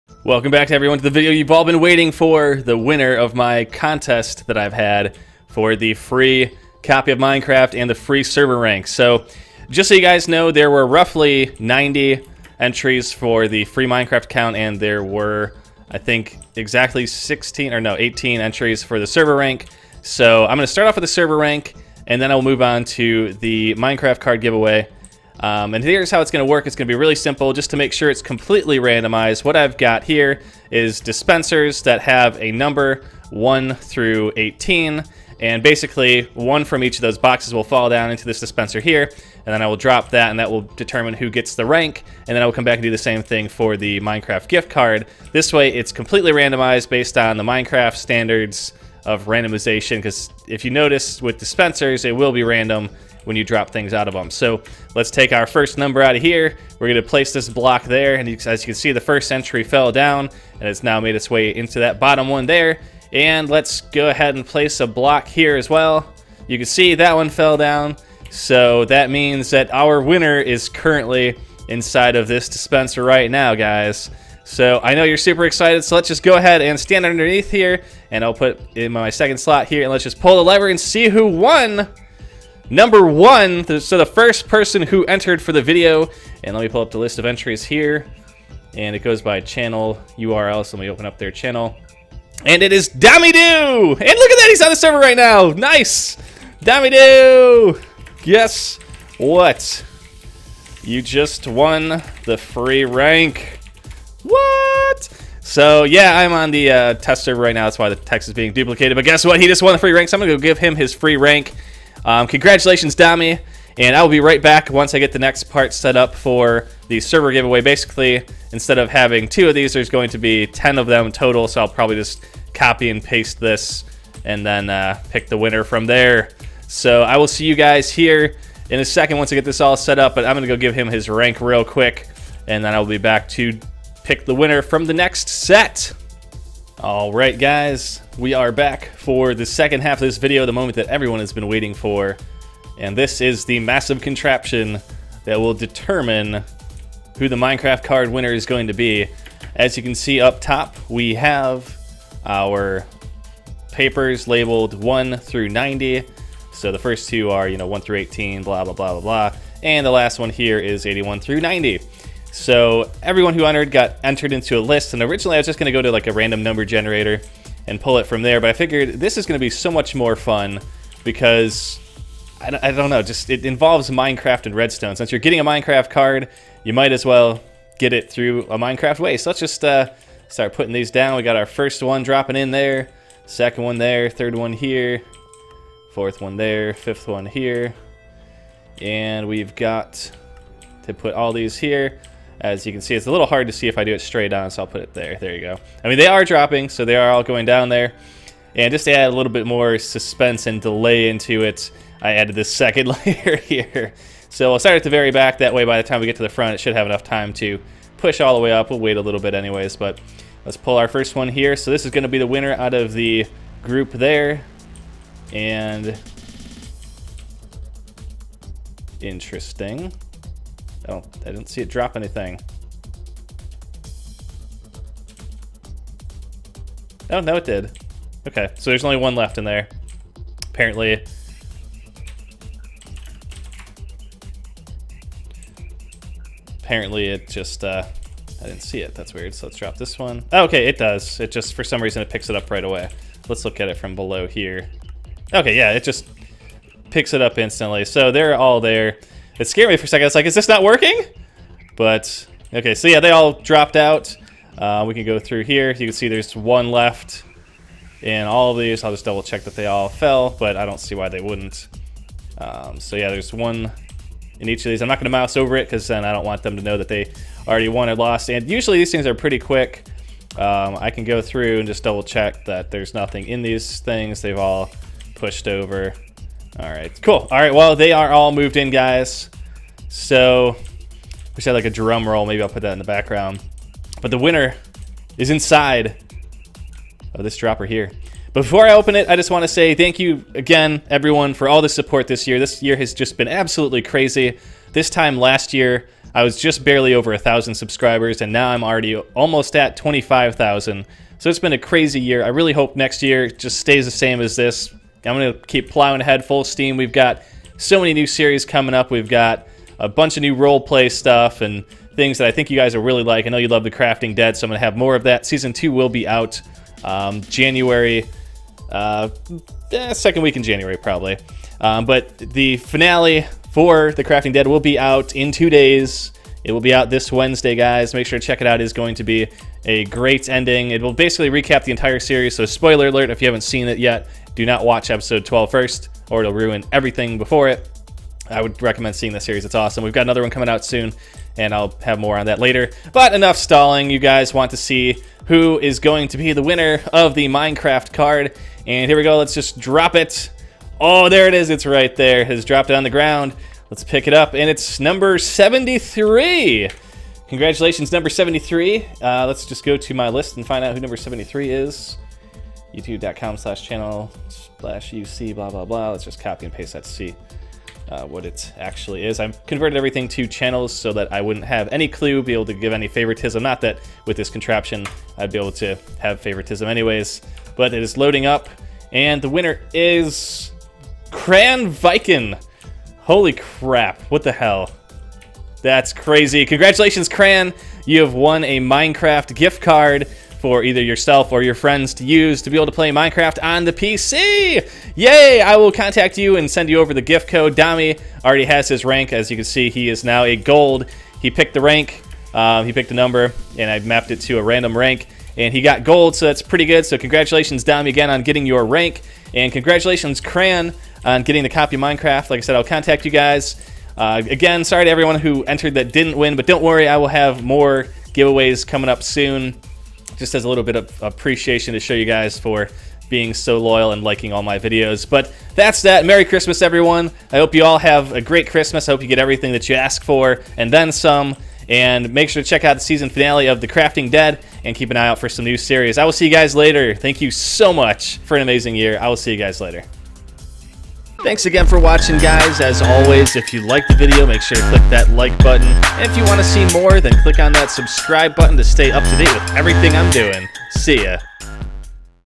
Christmas Giveaway Contest Winners Announced!
Congratulations to the winners of my contest! I wish I had enough stuff to give everyone something. Better luck in the future to those who didn't win.

Come play with me on my official minecraft server: hub.rgaminecraft.com

Add me on Steam or Skype: RGAMinecraft